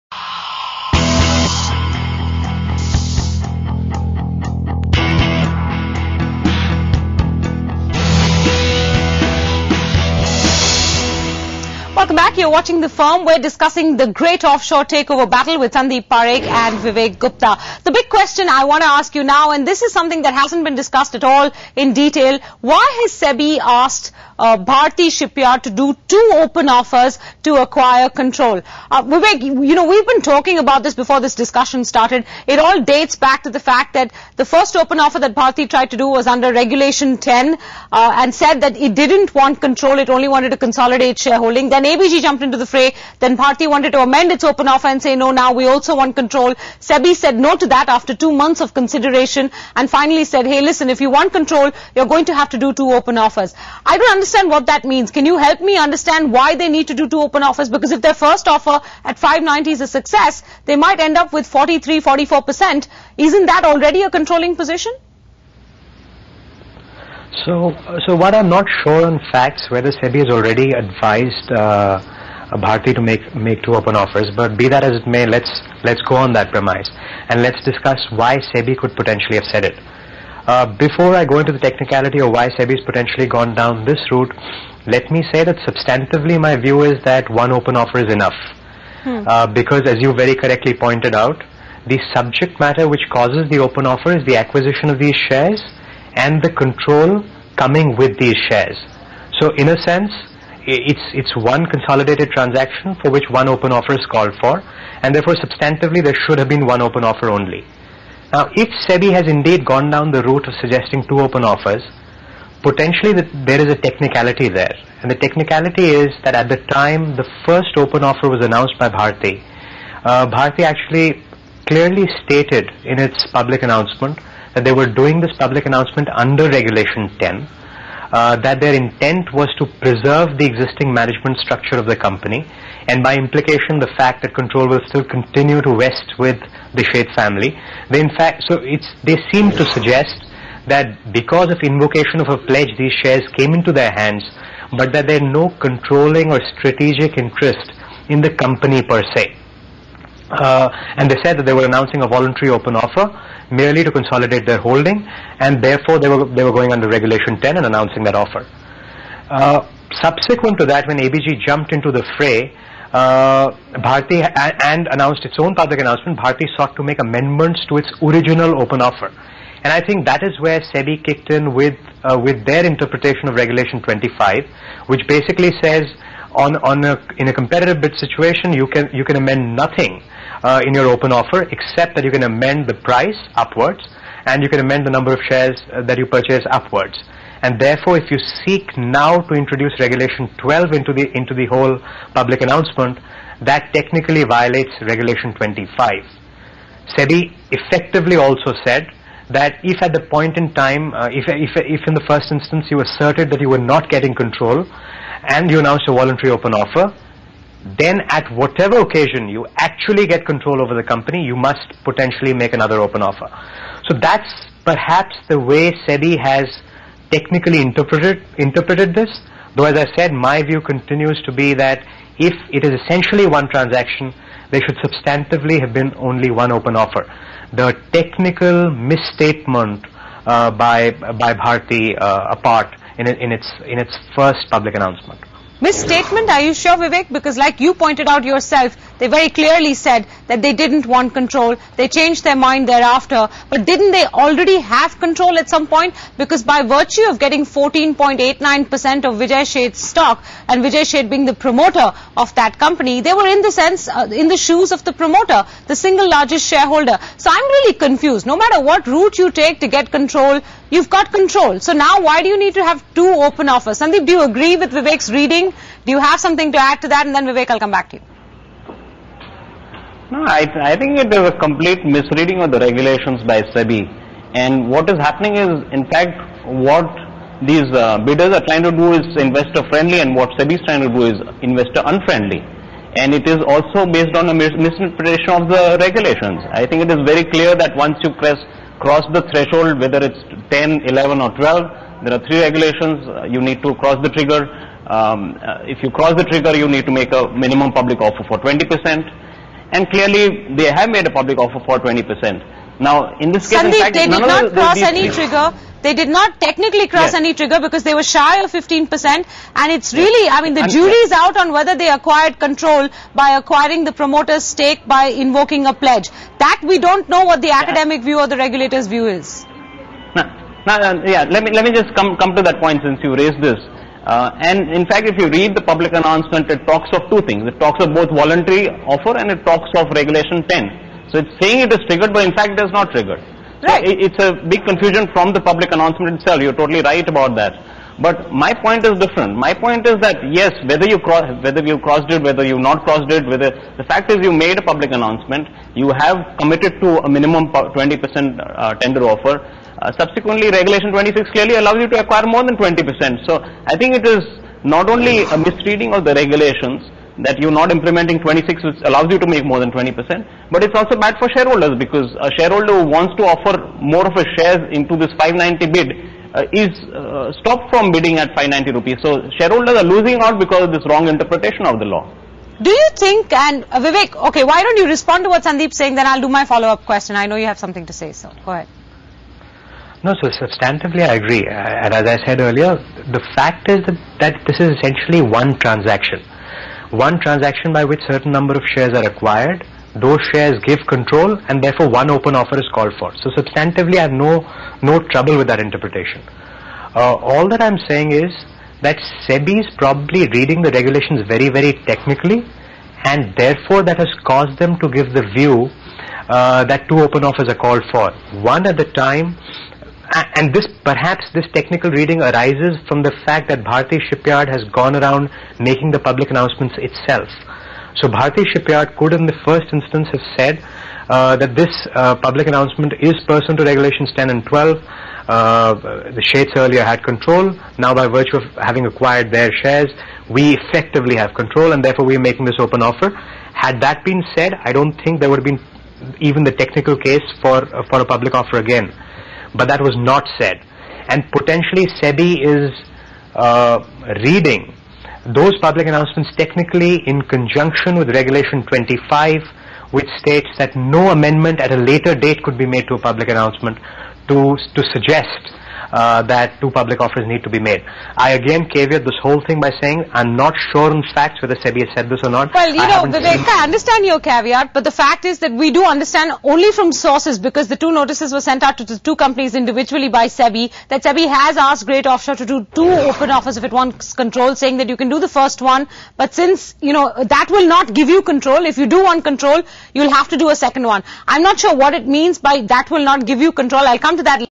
The Great Offshore Saga!
IIM A Professor Sandeep Parekh and BMR Advisors Partner Vivek Gupta discuss the Great Offshore saga. Whether ABG Shipyard was ethical in dropping out of the race and whether Bharati Shipyard should be required to do two open offers.      PART3